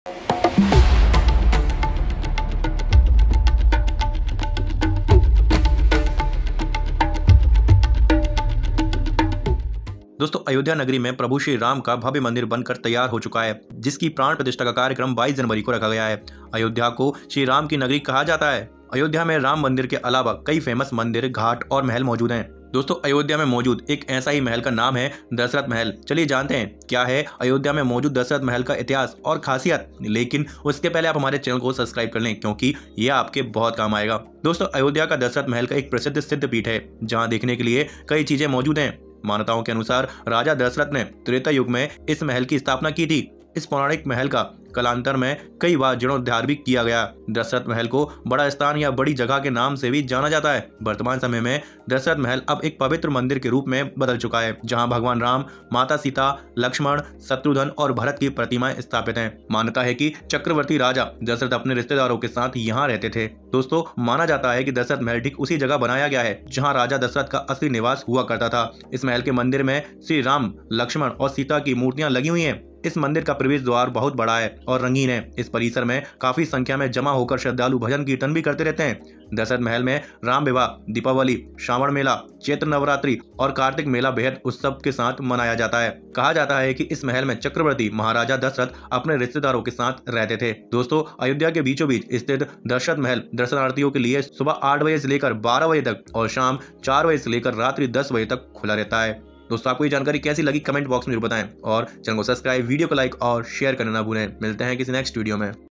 ये है भगवान राम जी के बचपन का महल राजा दशरथ महल
दोस्तो अयोध्या में 22 जनवरी को श्रीराम मंदिर की प्राण प्रतिष्ठा होनी है और अयोध्या प्राण प्रतिष्ठा के लिए पूरी तरह तैयार है लेकिन अयोध्या में राम मंदिर के अलावा कई और भी मंदिर और महल है उन्ही में से हमें है राजा दशरथ जी का महल दोस्तो वीडियो को पूरा जरूर देखे